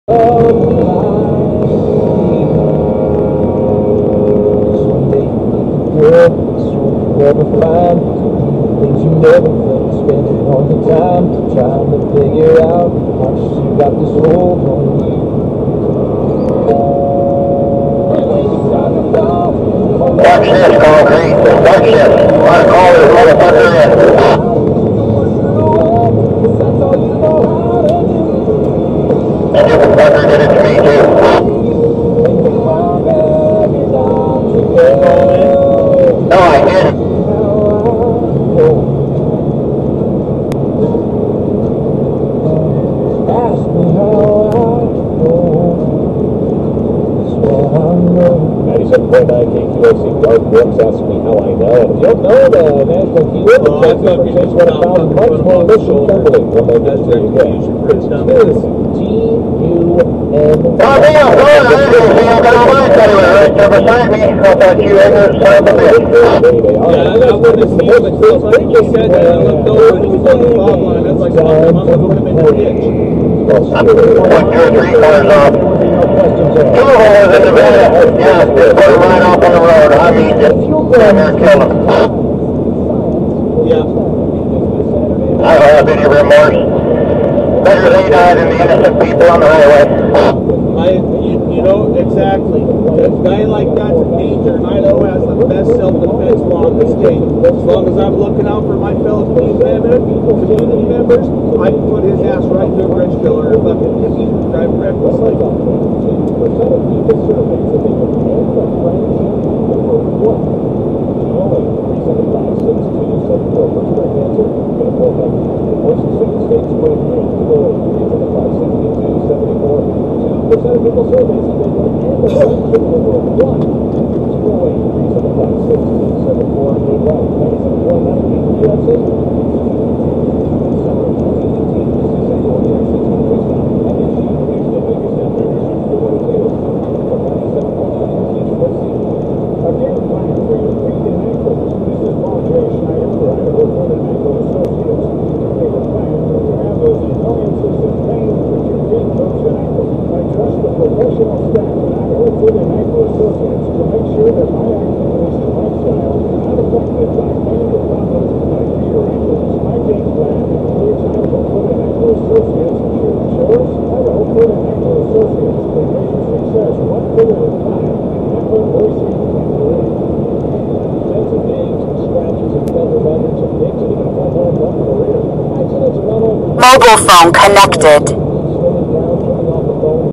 An Idiot Passing a Semi. Enough Said
Idiot passes oversized load on shoulder